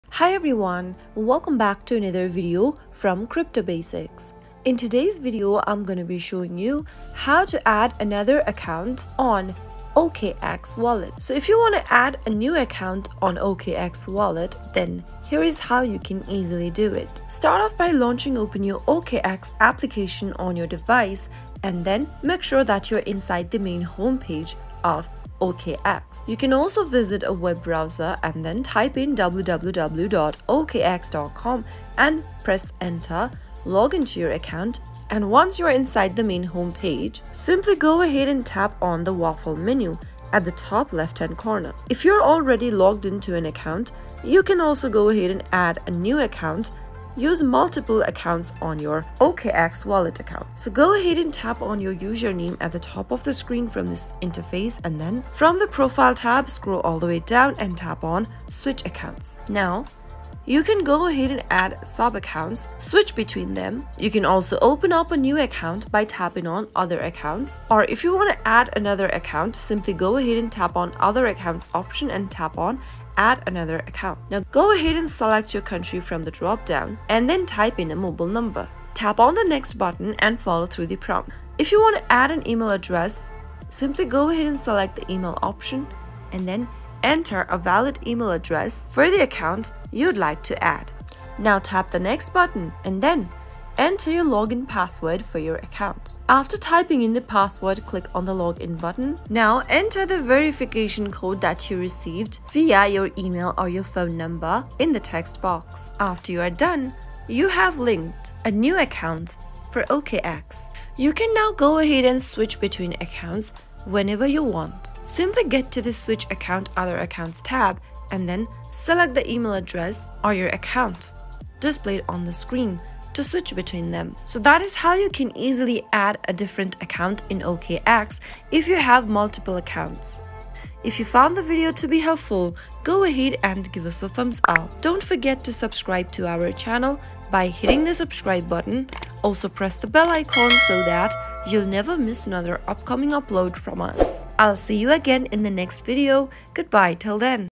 How to Add Another Account on OKX Wallet 2023?
Managing multiple cryptocurrency accounts can be a daunting task, but with OKX's user-friendly wallet, it's a breeze! In this step-by-step guide, we'll show you how to add another account on your OKX wallet, allowing you to seamlessly organize and manage your diverse crypto holdings.